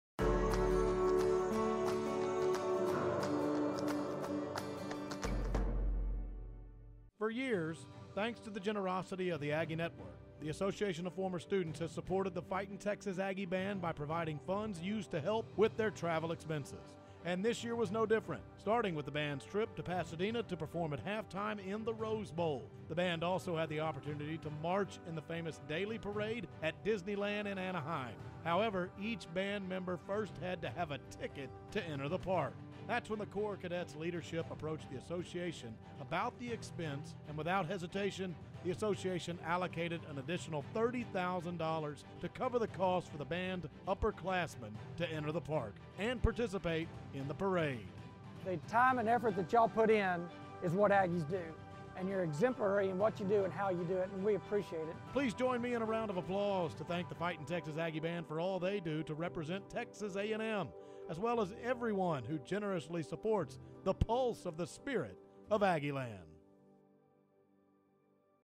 Century Club Impact Moment - Aggie Band Support
For years, thanks to the generosity of the Aggie Network, The Association of Former Students has supported the Fightin’ Texas Aggie Band by providing funds used to help with their travel expenses.

And this year was no different, starting with the band’s trip to Pasadena to perform at halftime in the Rose Bowl.

The band also had the opportunity to march in the famous daily parade at Disneyland in Anaheim; however, each band member first had to have a ticket to enter the park.

That’s when the Corps of Cadets leadership approached The Association about the expense and without hesitation, The Association allocated an additional $30,000 to cover the cost for the band upperclassmen to enter the park and participate in the parade.